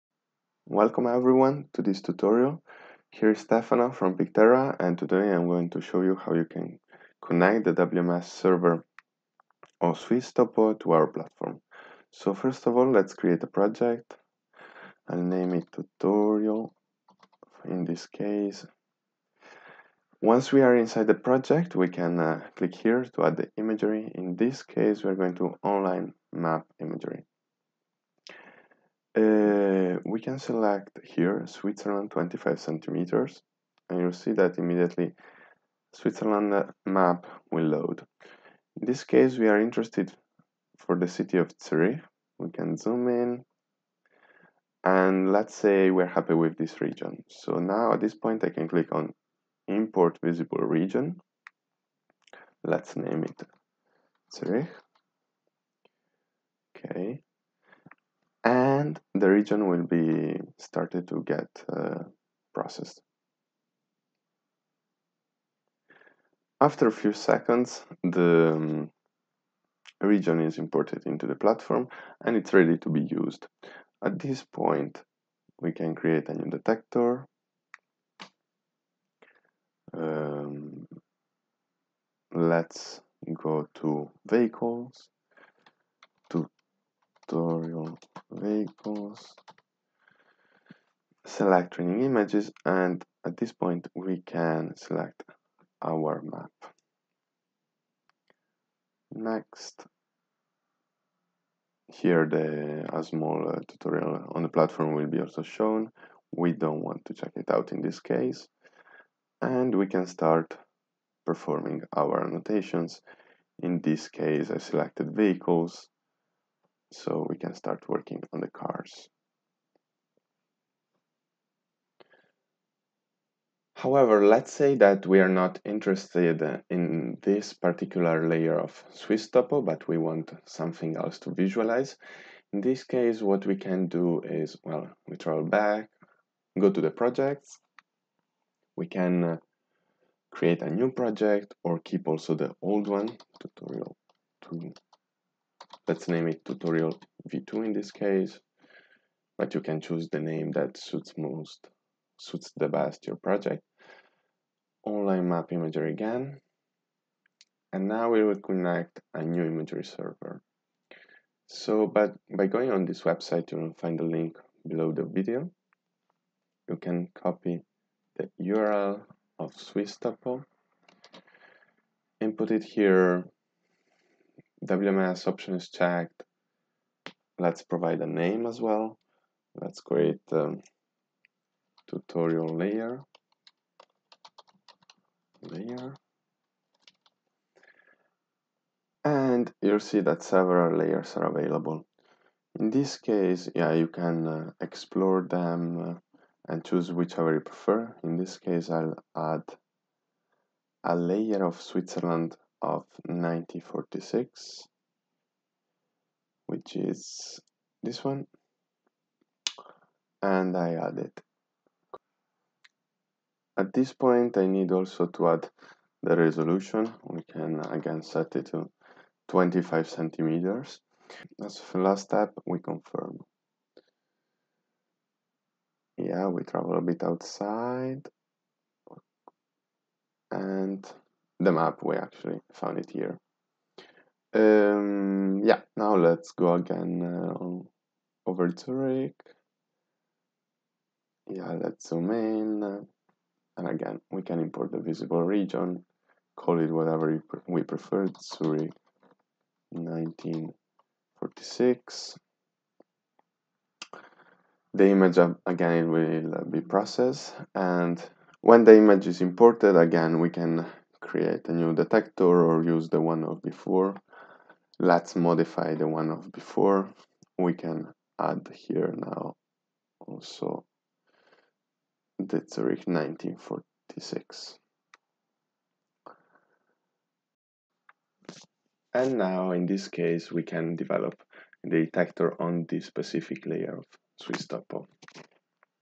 Swisstopo WMS tutorial - Picterra
Learn how to connect Swisstopo WMS service to the Picterra platform and thus get valuable geospatial insights.

The WMS-FSDI is accessible via a secure connection. The URL is